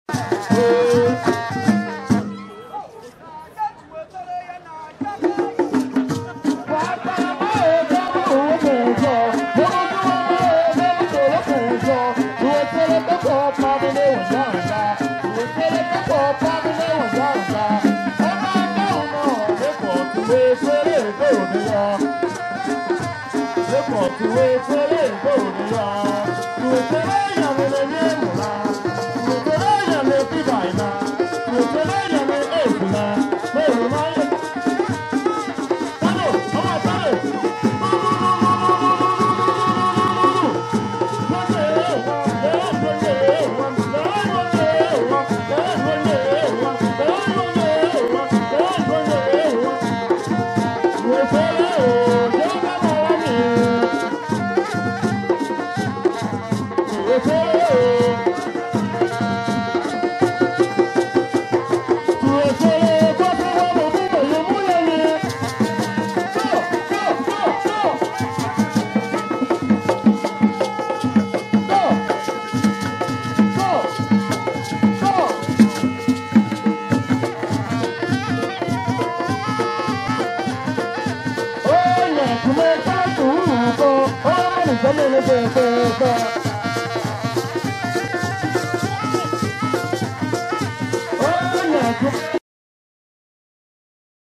THE BEST TIV DANCE....A K A CAT DANCE
The best Tiv dance cat dance
Tiv people have the best dance in the world